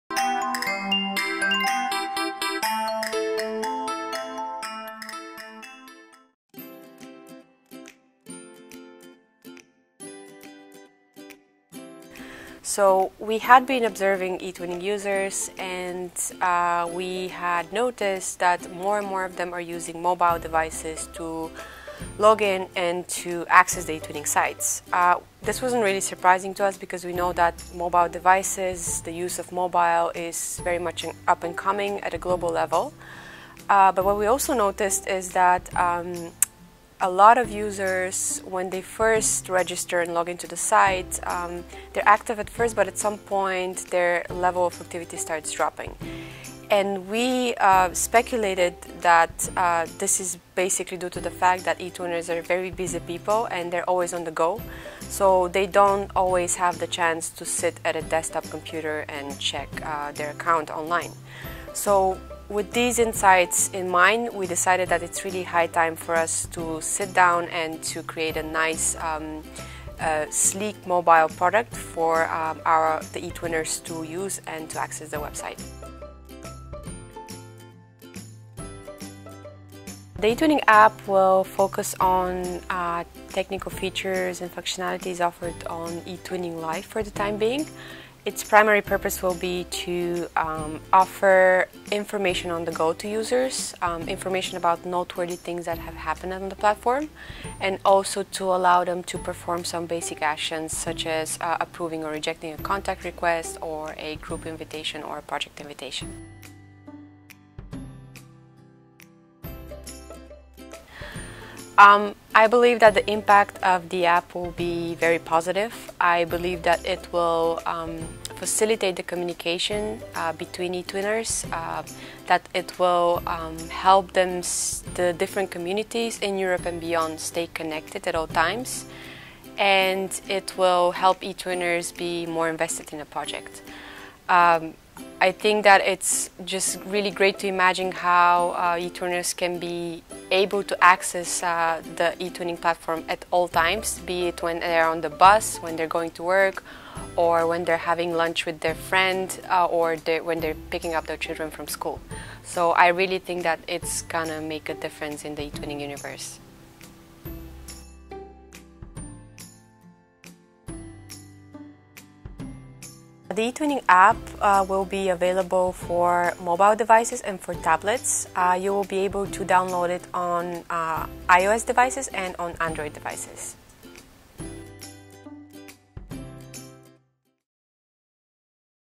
eTwinning App - Denitsa Gospodinova
eTwinning is going mobile! Watch Denitsa Gospodinova, Digital Officer of EUN, explain thoroughly the functionalities, impact and the properties of the app in this wonderful interview.